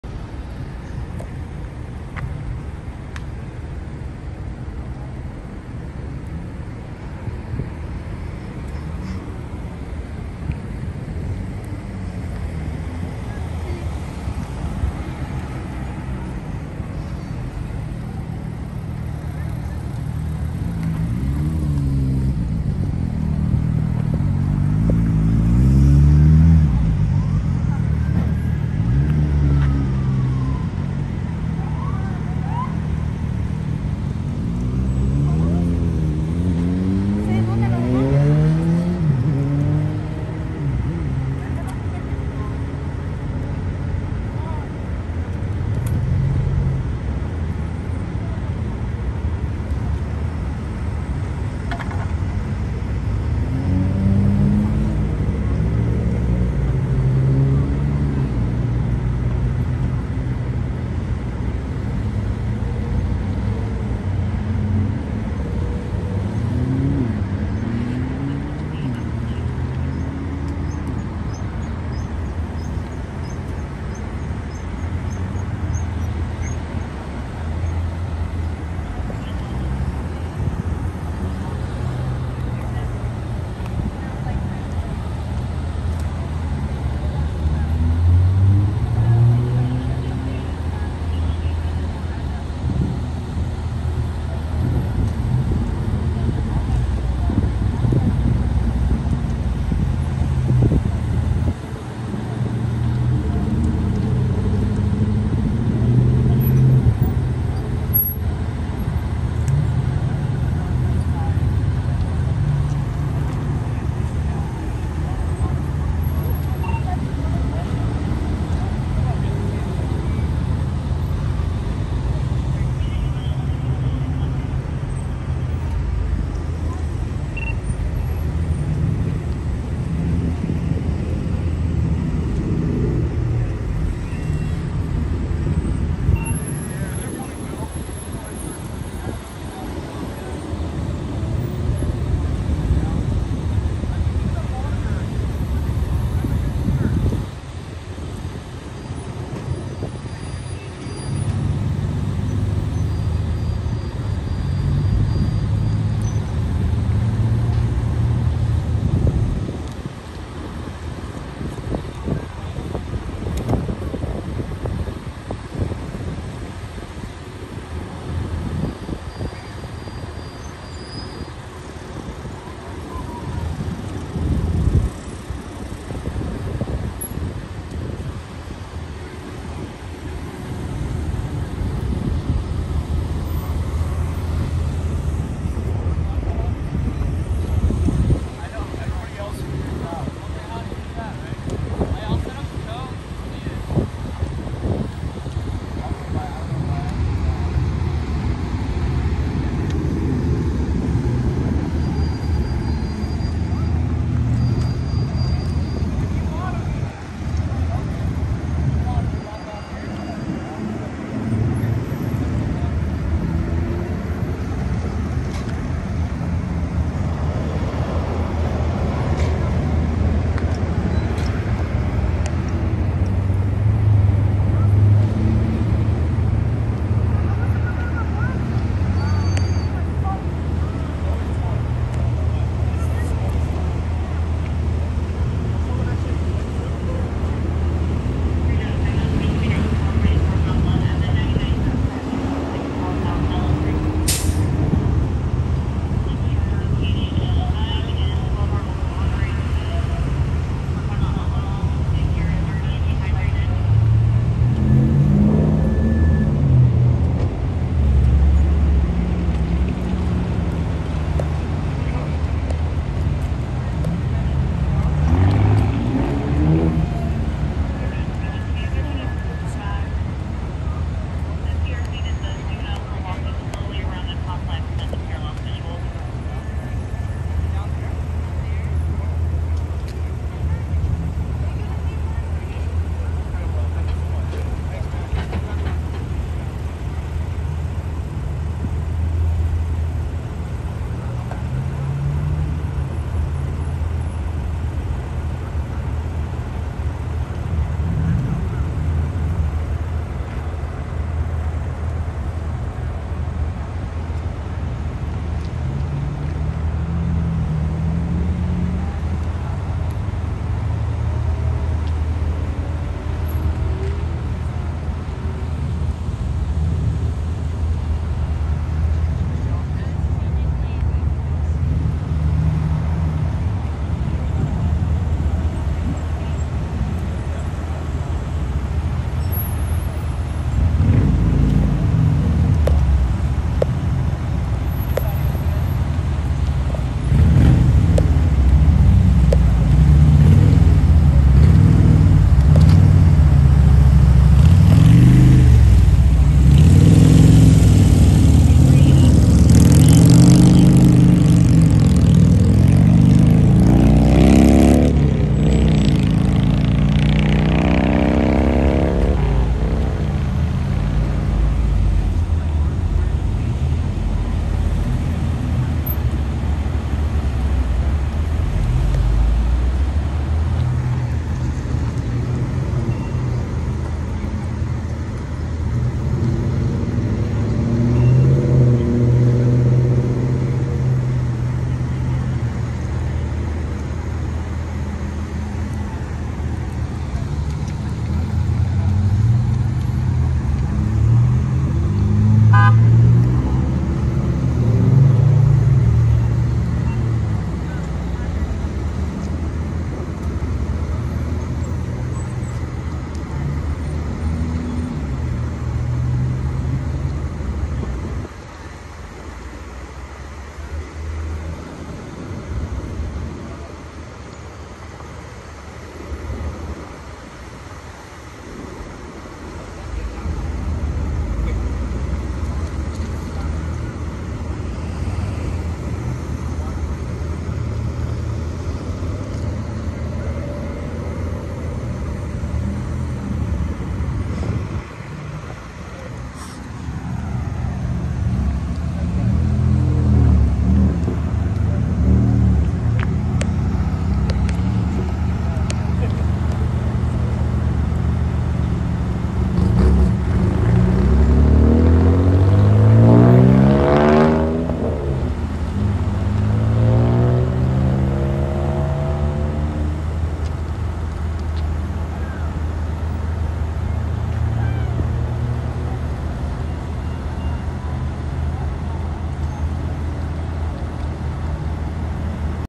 ROLLOVER CRASH TROPICANA & PECOS 05/21/2024 LAS VEGAS
After Hearing a Loud Crash

This is what occurred